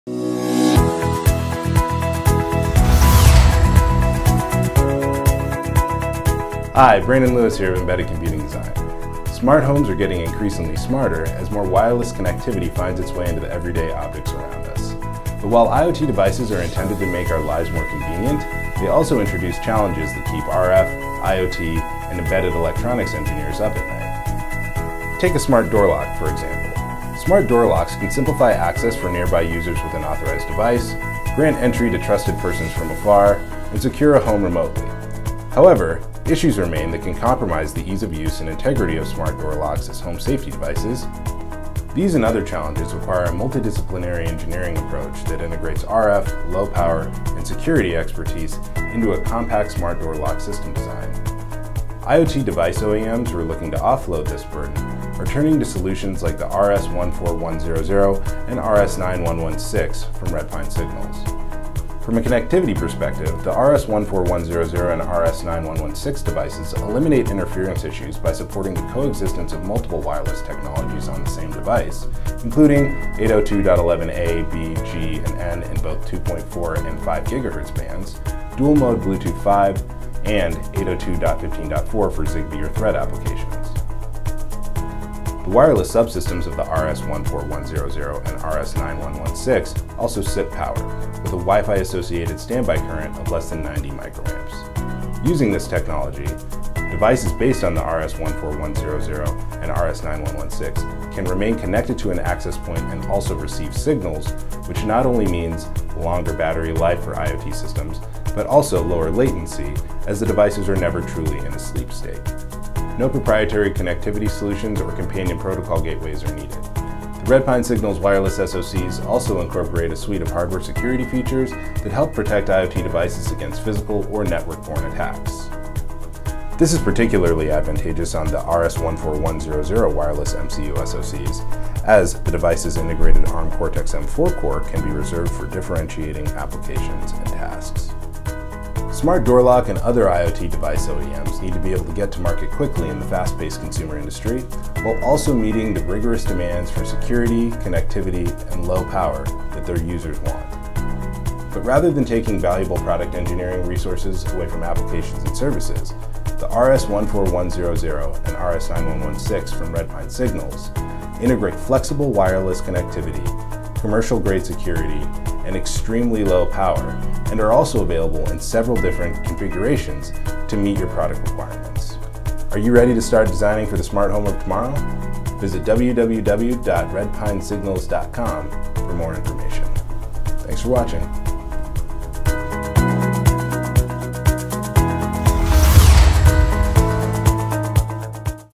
Redpine Signals – Low-Power Multi-Protocol Wireless Solutions for Secure Smart Homes
While they may seem simple to users, IoT devices like smart door locks are packed with complexity. Wireless interference, security, and power consumption are just a few of the challenges that IoT design engineers must overcome in a smart door lock design. But while these are critical elements of the system, they also don’t add value in the eyes of consumers who expect smart door locks to “just work.”

Redpine Signals RS14100 and RS9116 Multi-Protocol Wireless MCUs and SoCs integrate wireless coexistence capabilities, built-in commercial grade security, and an absurd Wi-Fi associated standby current of less than 90 microamps. IoT device OEMs can integrate these devices in smart door lock and other smart home device designs so that engineering resources can focus on adding differentiating features and getting to market quickly.